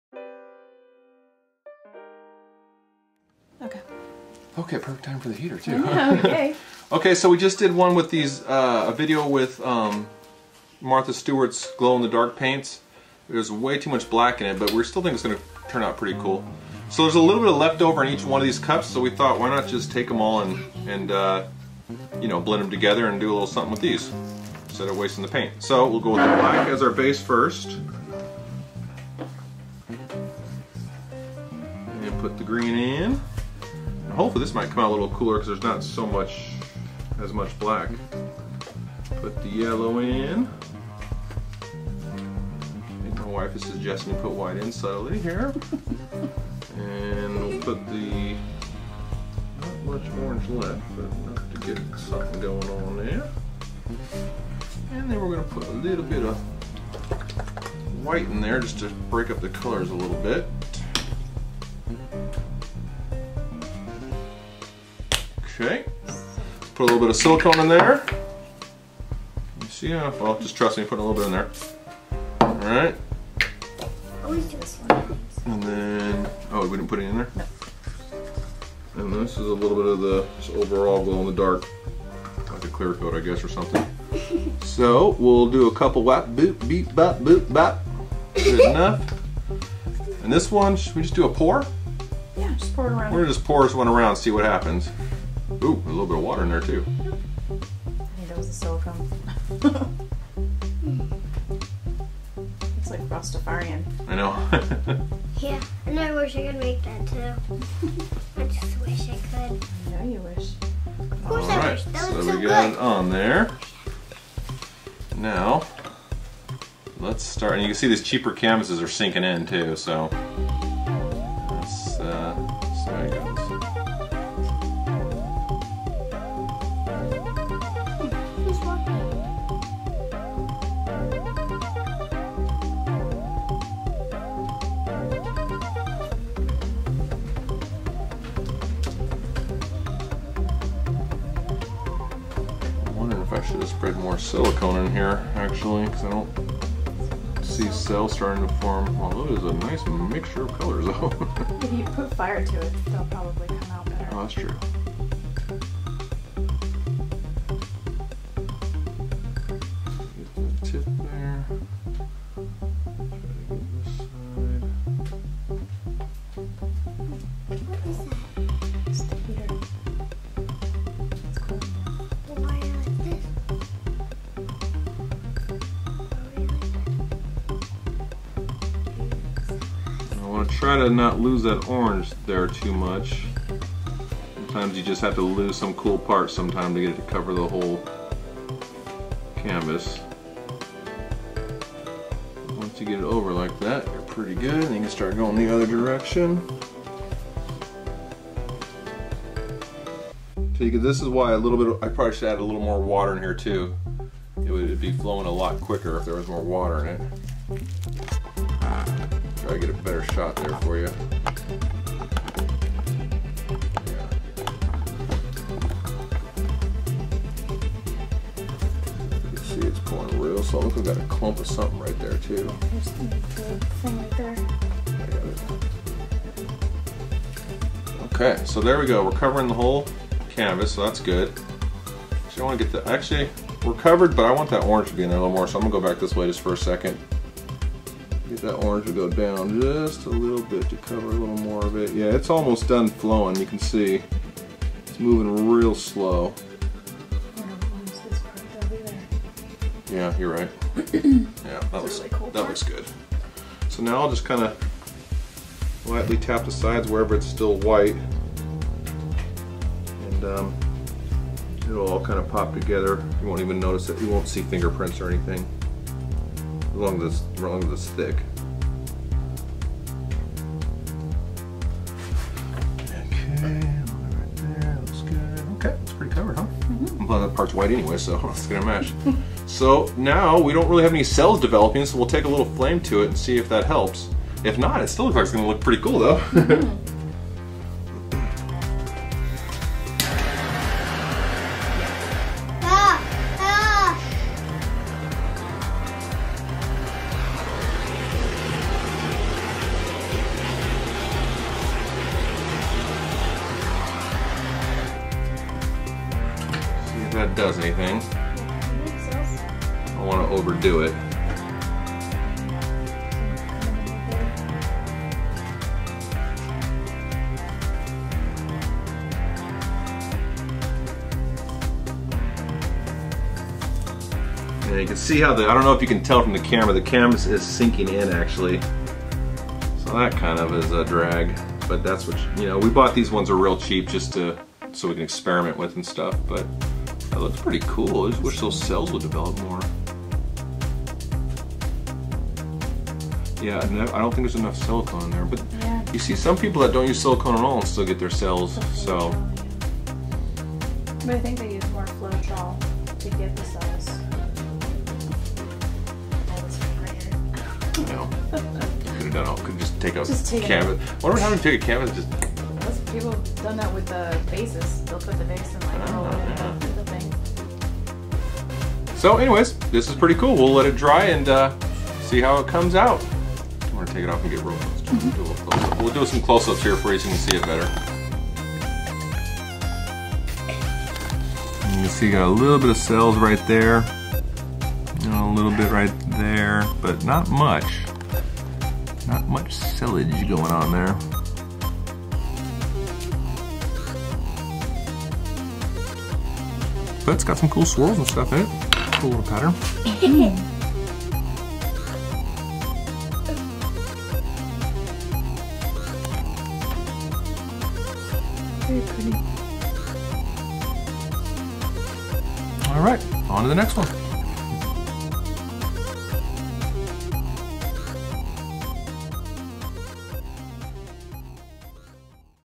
Acrylic Pour 6 x 6 canvas (left over acrylic paint)
We had a little paint left over from our Outer Space pour and we have a bunch of 6" x 6" canvas's for just this reason. Check it out and let us know what you think.